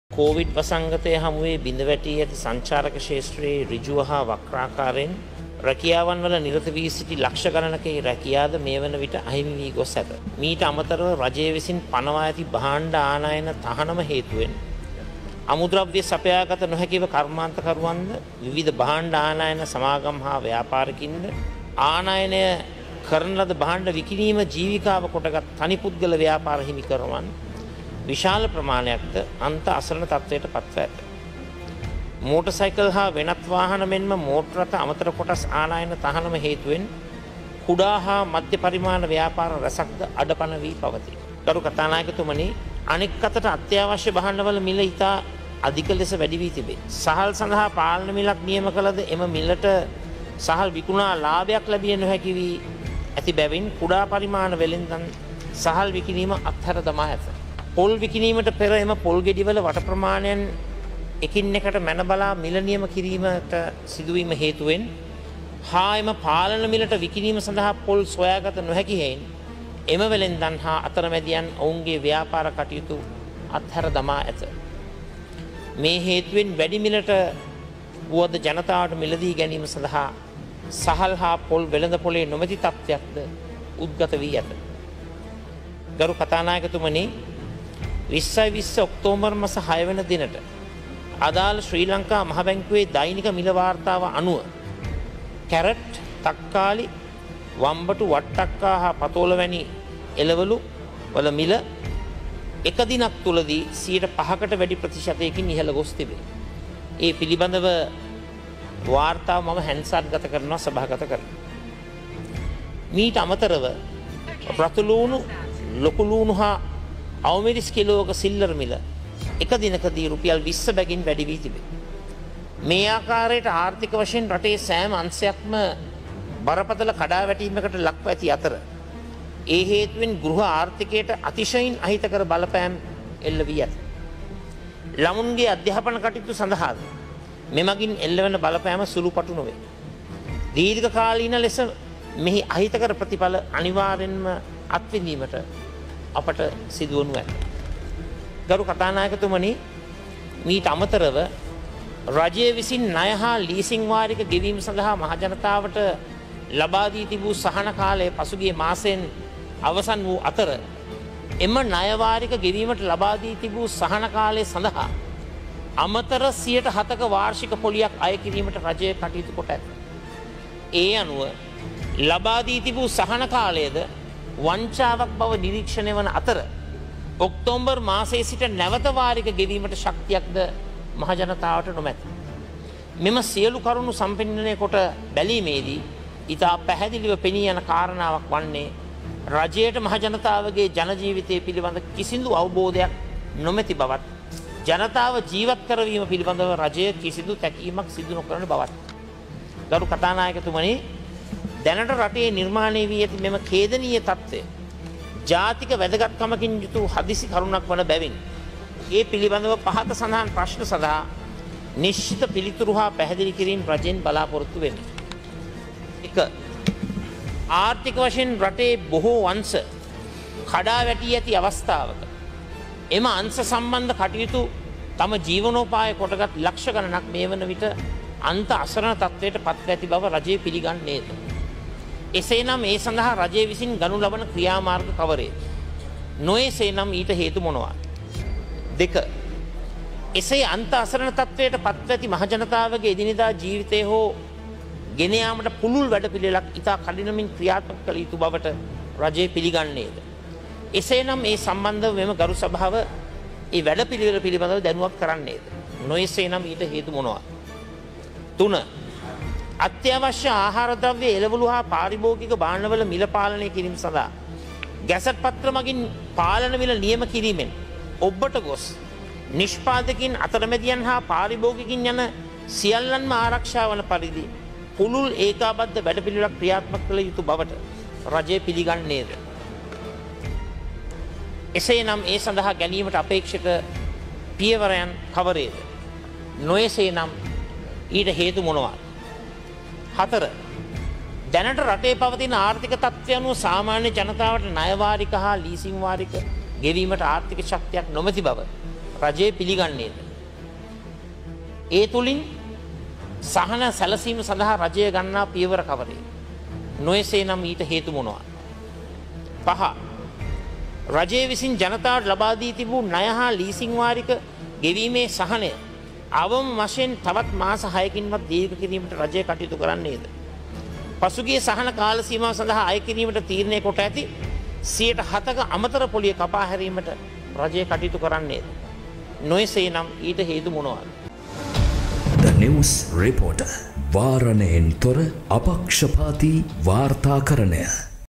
ජන ජීවිත කබලෙන් ලිපට - ආණ්ඩුවට සජිත්ගෙන් ප්‍රශ්න පත්තරයක්
ජන ජීවිත කබලෙන් ලිපට වැටිලා - විපක්ෂ නායක සජිත්ගෙන් පාර්ලිමේන්තුවේ දී ආණ්ඩුවට ප්‍රශ්න රැසක්.